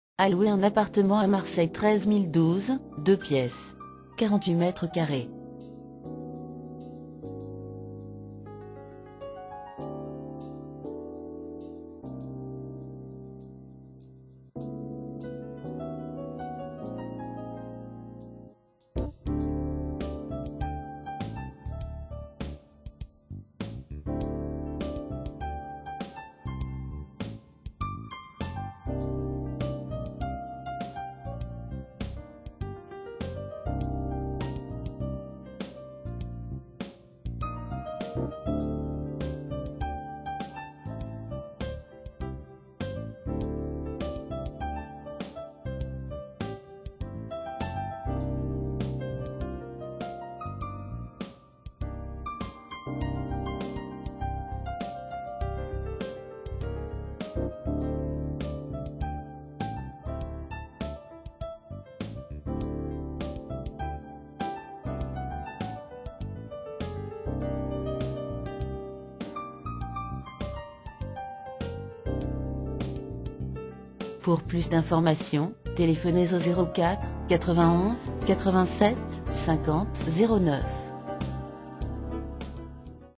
Location Appartement MARSEILLE 13012 13012 - 48 m2
Cliquez sur ce lien pour en savoir plus (prix, contacts)

A louer à MARSEILLE 13012 un appartement de 48 m2.

L agence Solvimo Marseille 11-12 vous propose de nombreux autres biens immobiliers.

Annonce immobilière : Location Appartement MARSEILLE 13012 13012 Bouches du Rhône